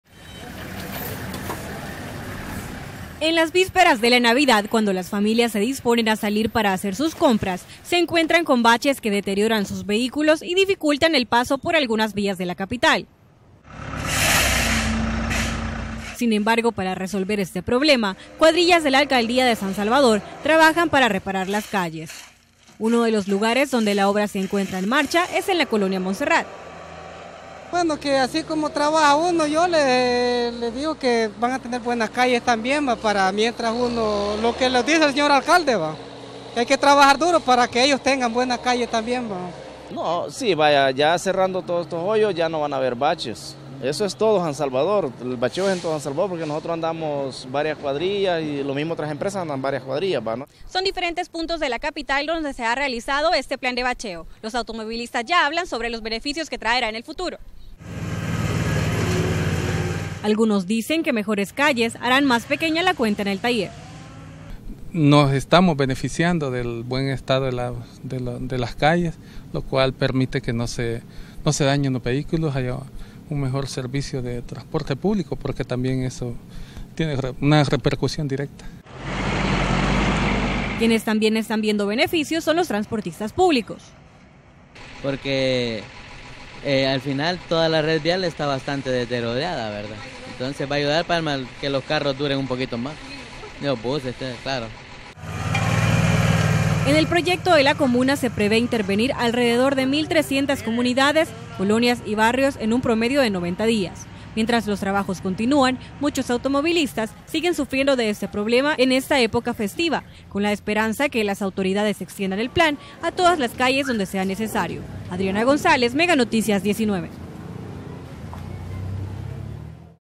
Cuadrillas de la Alcaldía de San Salvador trabajan en la mejora de calles
Informa Adriana González